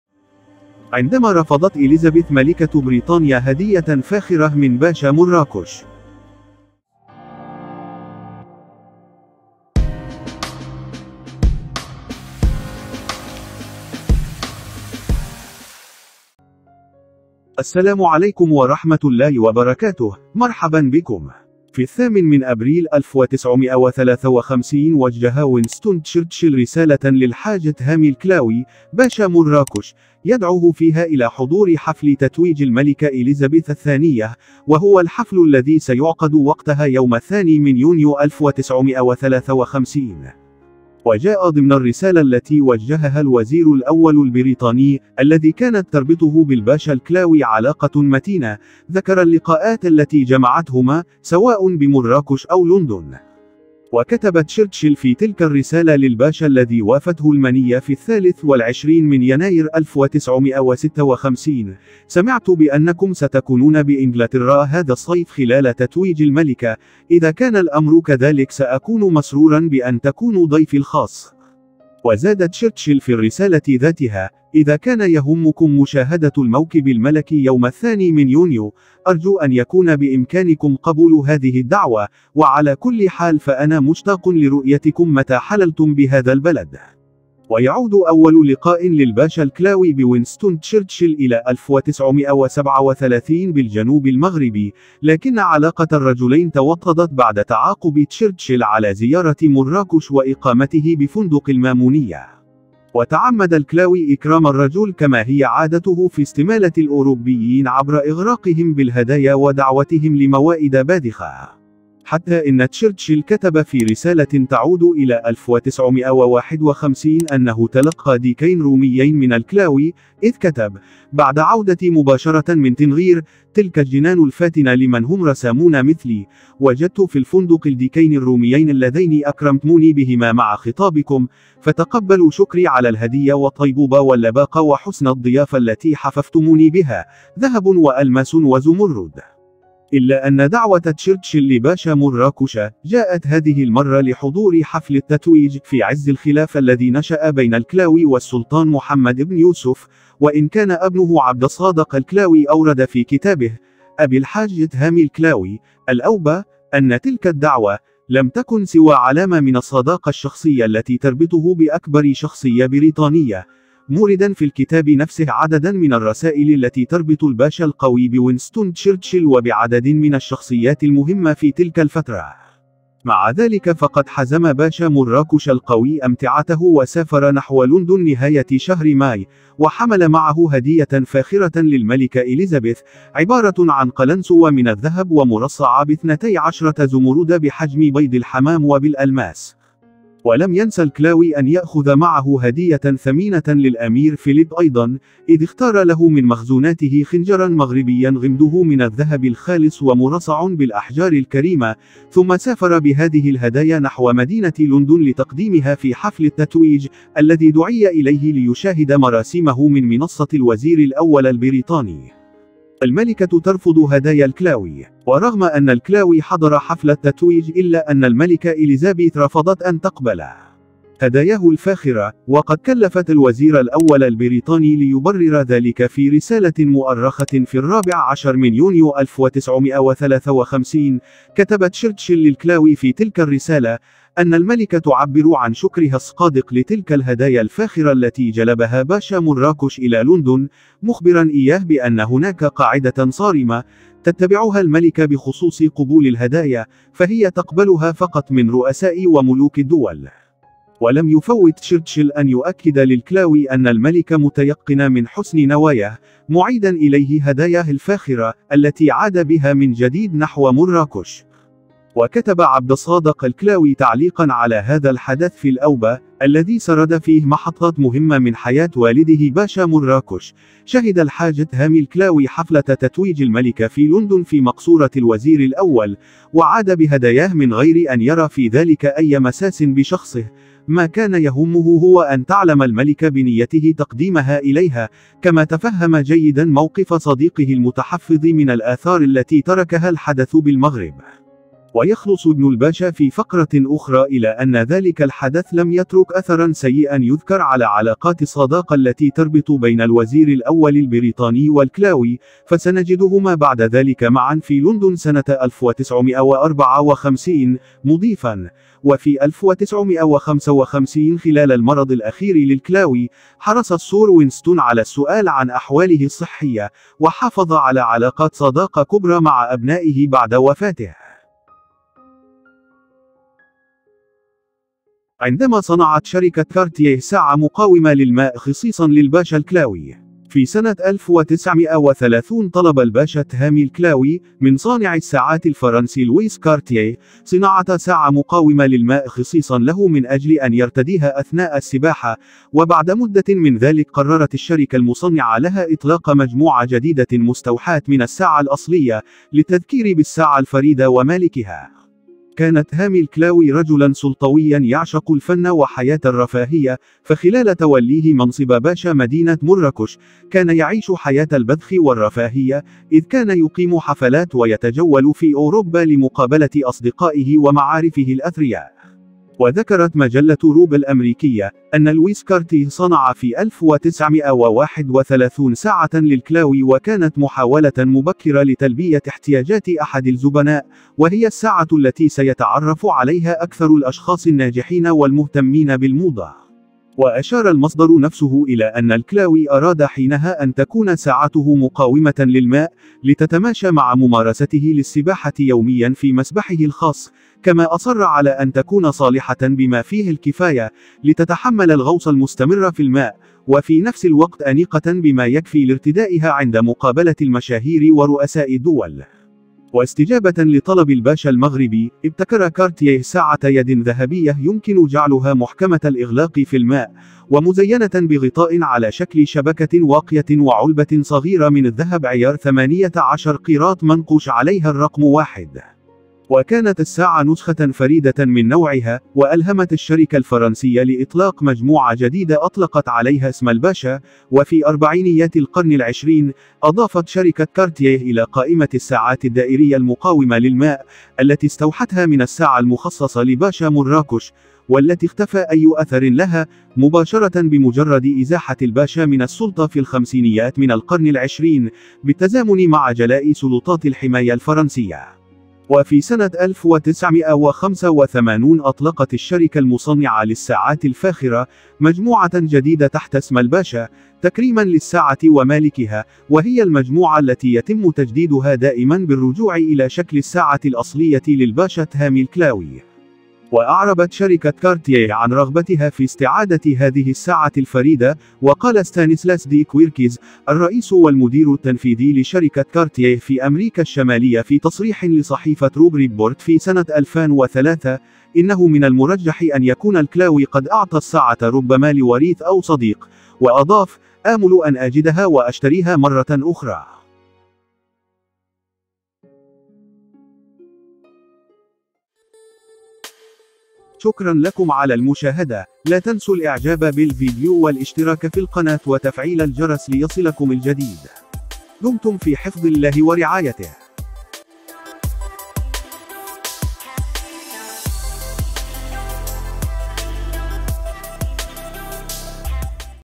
عندما رفضت الملكة اليزابيث هدية فاخرة من باشا مراكش التهامي الكلاوي
مـورونيـوز®️ قناة اخبارية مغربية شاملة